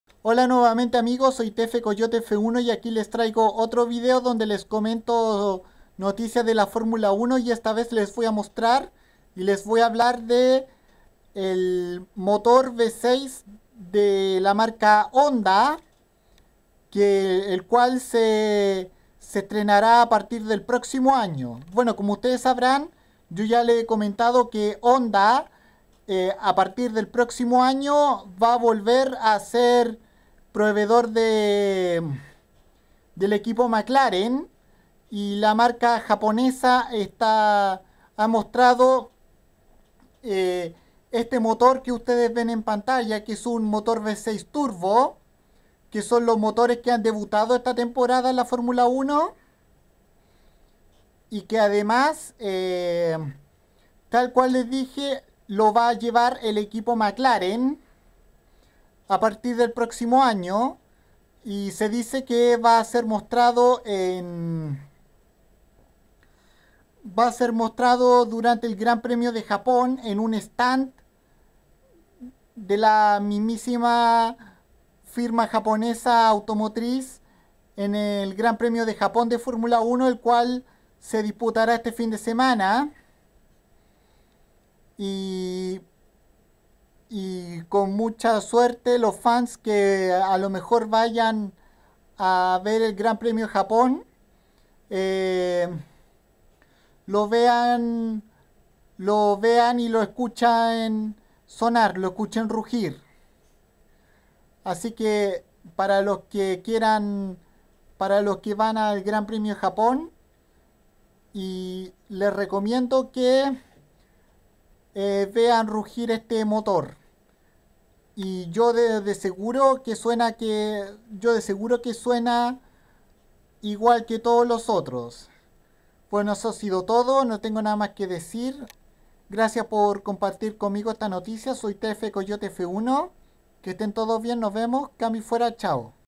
Honda presenta su motor V6 Turbo de F1 para 2015
Para el GP de Japón que se correrá este fin de semana la marca Honda presentará al público su motor V6 Turbo de 2015 el cual lo llevará el equipo Mclaren para la próxima temporada